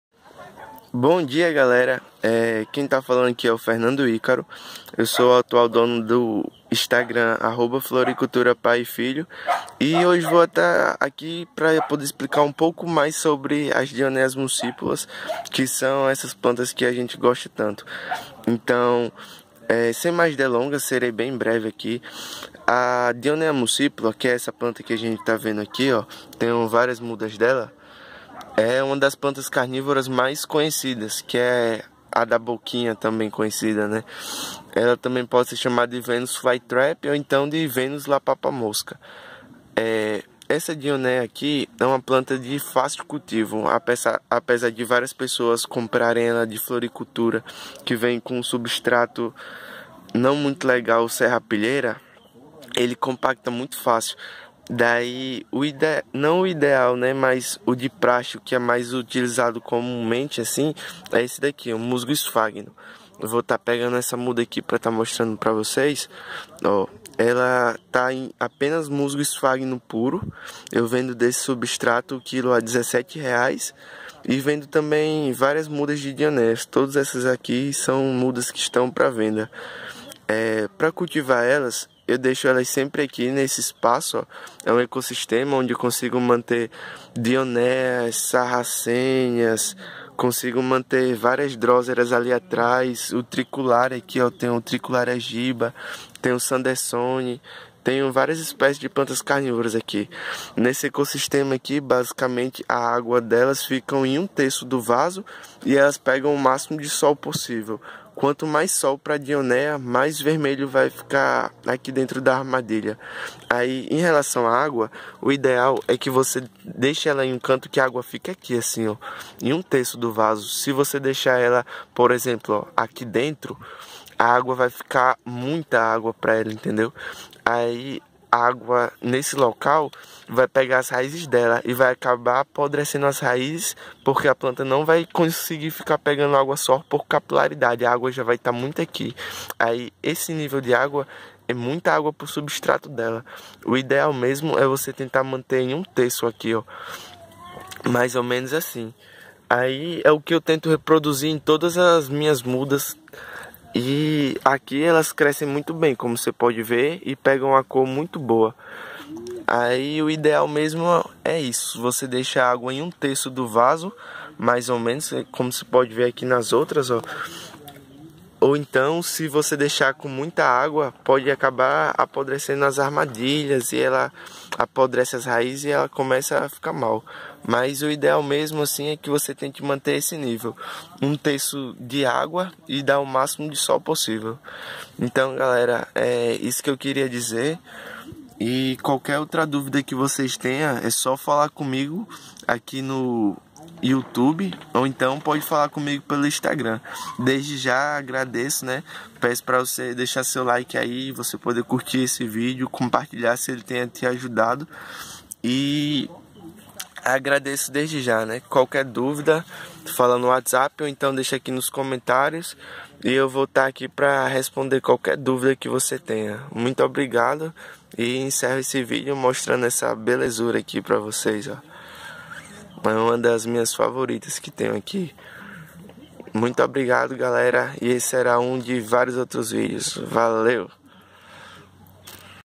Como Cultivar as Plantas Carnivoras Dionaea Muscipula Venus Fly Trap
Como Cultivar as Plantas Carnivoras Dionaea Muscipula Venus Fly Trap de uma maneira fácil e sem gastar muito - LEIA A DESCRIÇÃO
. Iluminação: minimo 4h de sol direto na planta para mante-la viva, quanto mais sol para ela mais ela cresce.
. Rega: água sempre por baixo do vaso em 1/3
. Substrato: musgo sphagnum